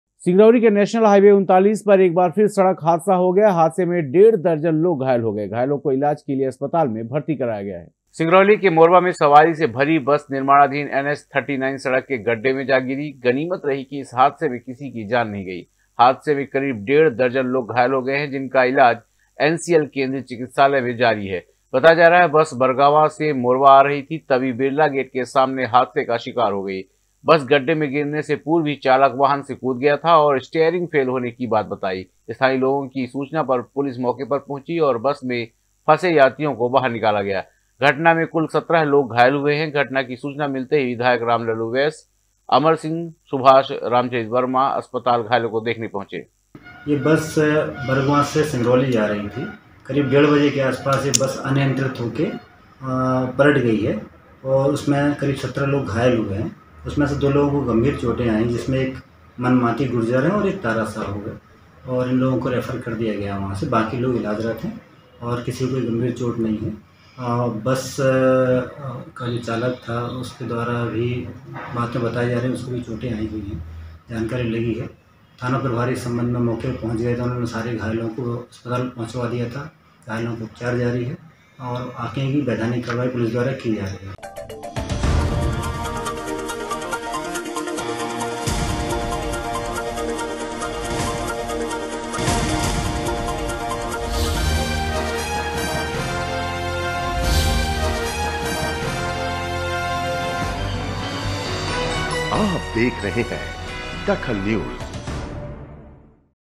DAKHAL NEWS NH - 39 में फिर हुआ सड़क हादसा
NH - 39 में फिर हुआ सड़क हादसा
बस से यात्रियों को बाहर निकाला गया